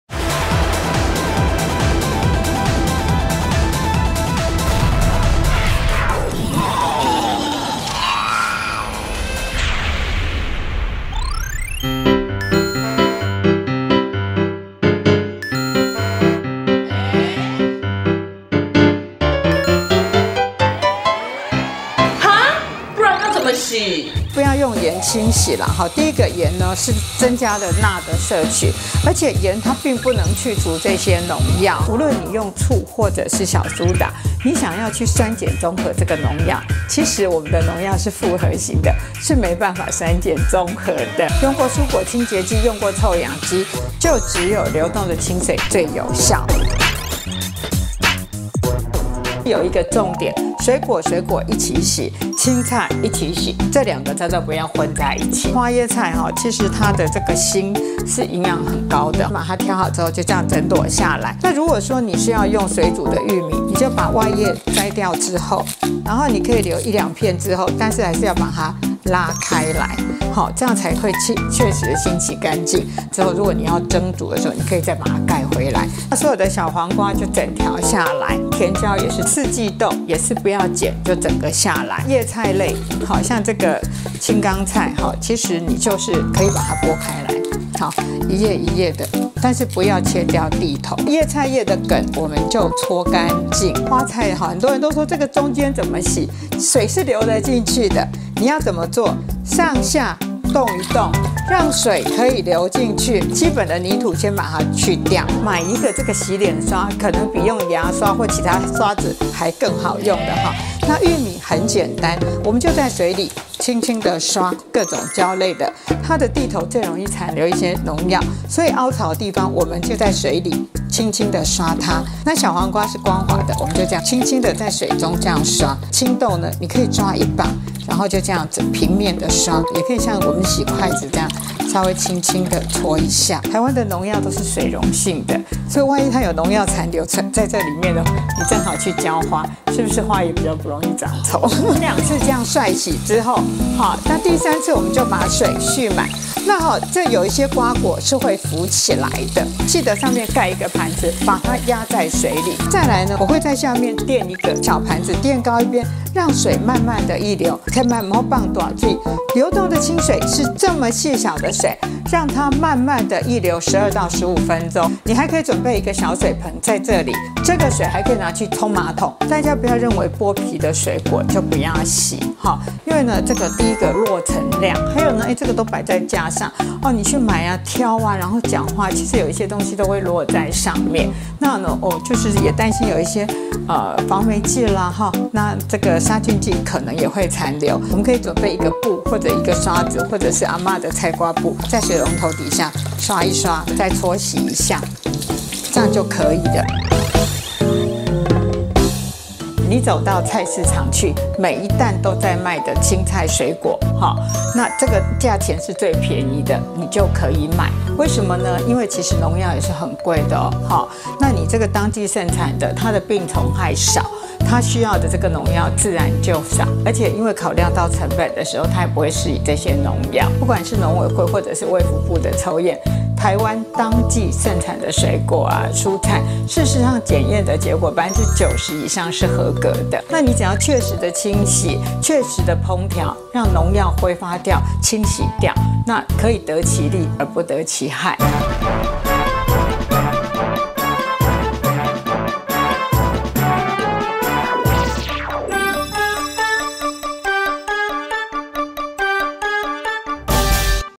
破除蔬果清洗的不實謠言！蔬果怎麼洗才安心？｜科學大爆炸EP.41
吃橘子你從來沒有洗過？青江菜、花椰菜你都是切了才洗？洗菜你會加鹽巴避免農藥殘留？千萬嘸通啊！

那要怎麼避免農藥殘留？其實很簡單！最有效的是流動清水，再加上一些小撇步，就能讓我們遠離農藥殘留。跟著譚敦慈護理師一起這樣做吧！